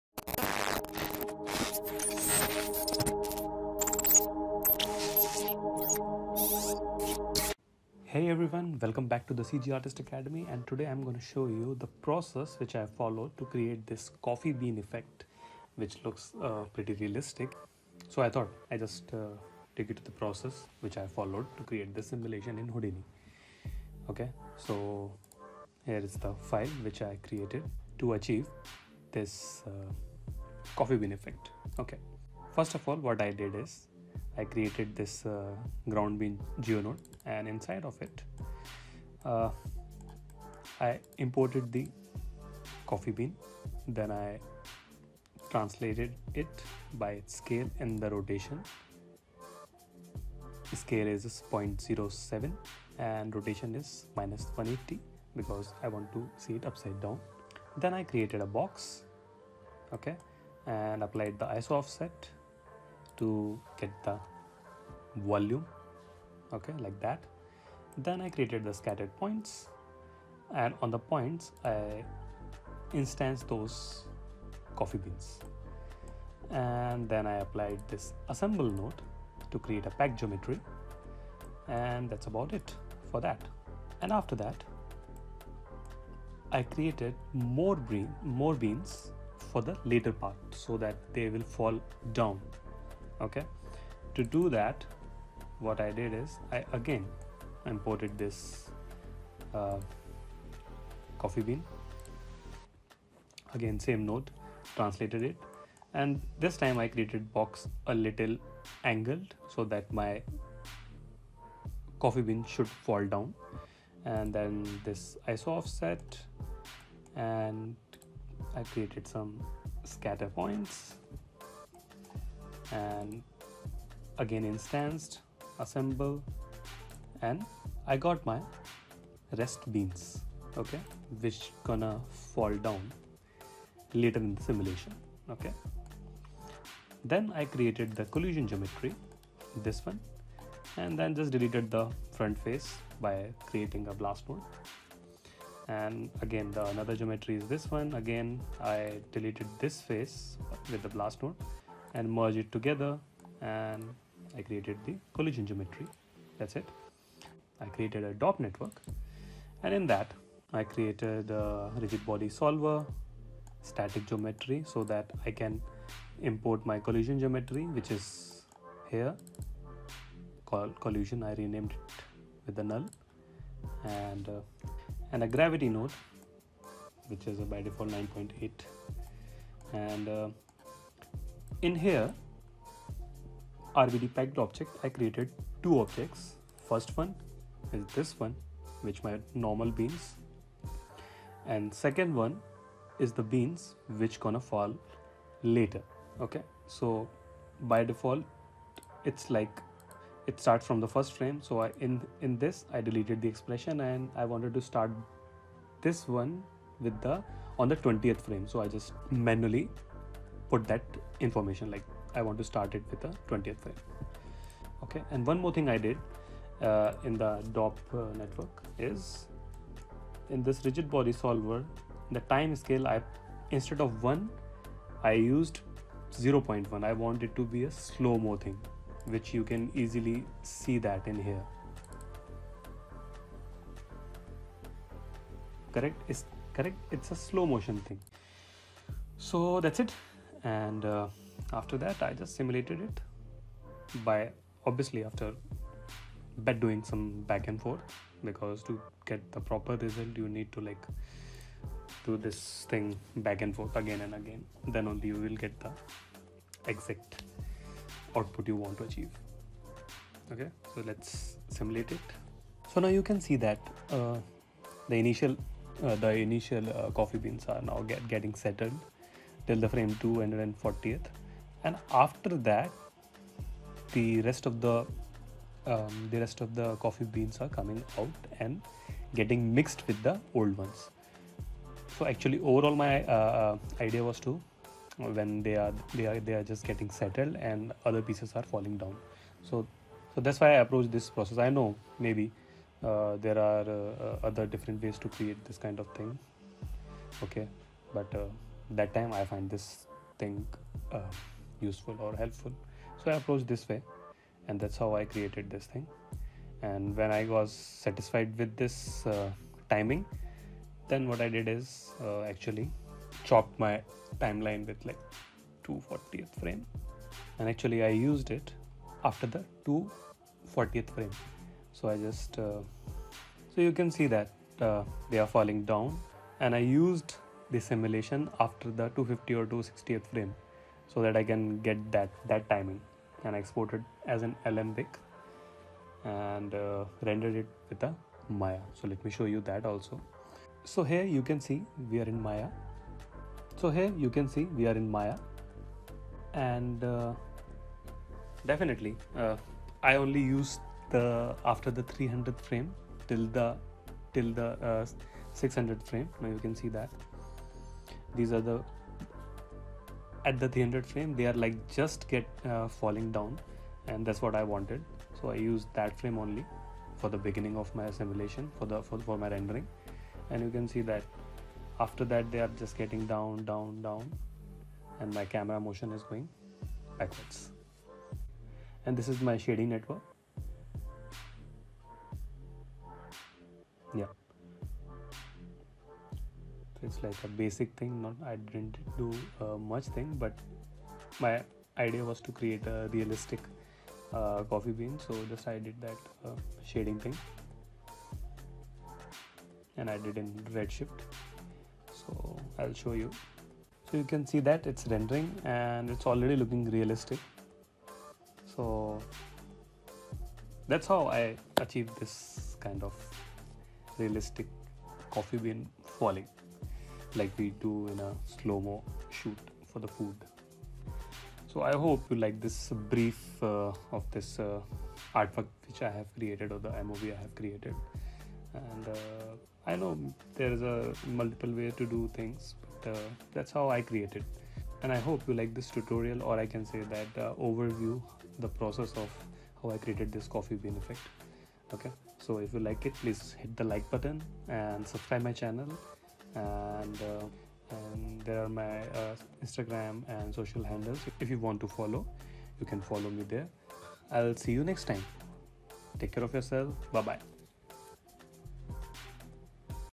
Houdini Coffee Bean Simulation Tutorial (Project files Included)
In this tutorial, I will show you the process of how I set up my scene and simulated in Houdini. Its an overview of how I achieved the desired simulation. Again there are multiple ways to achieve the output so if you guys have some other or faster way to get this kind of output let me know in the comments below.

Thanks